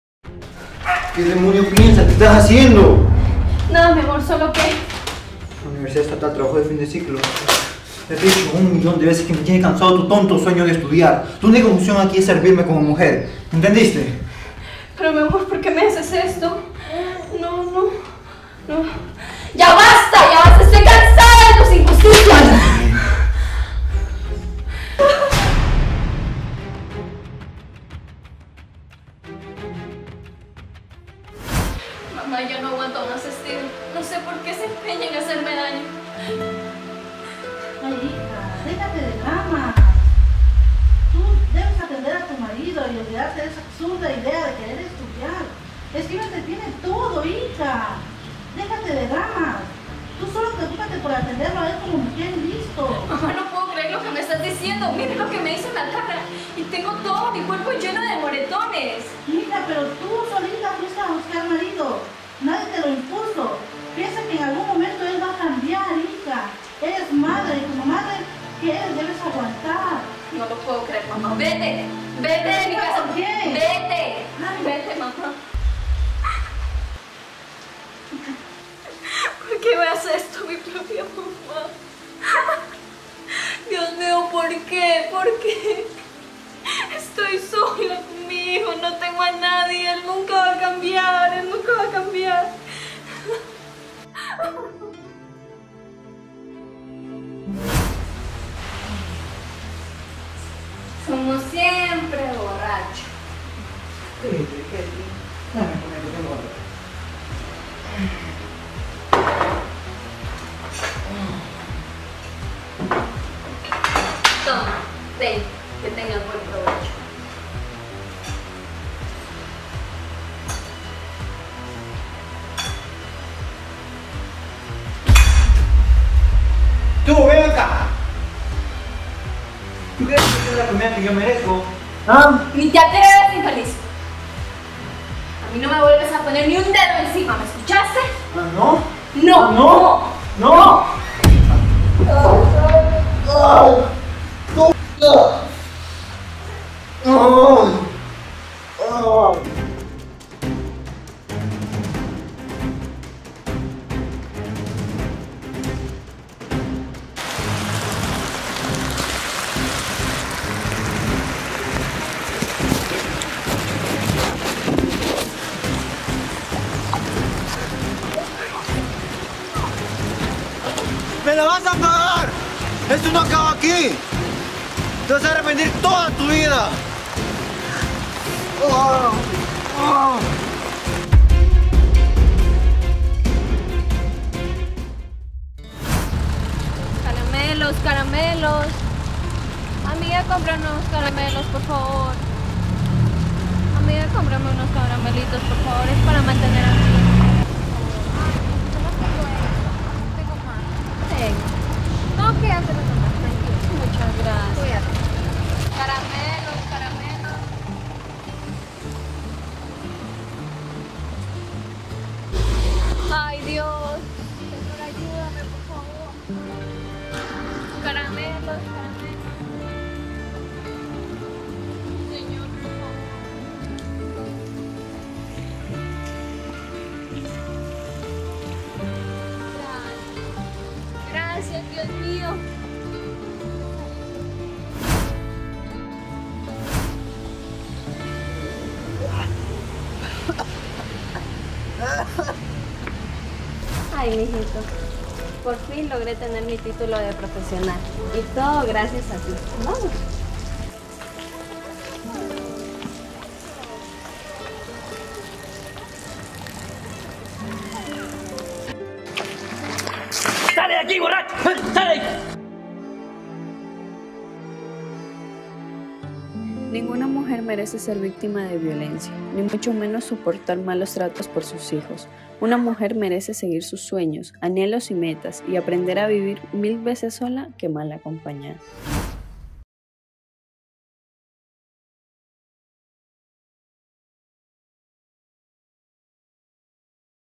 Madre ama de casa lucha por ser profesional, pero su esposo bσrrachσ se lo prohíbe.
Hola chicos soy Blad!🥰
Bienvenidos a mi nuevo Canal!🥳
Aquí subiré mis nuevos Videos!🤩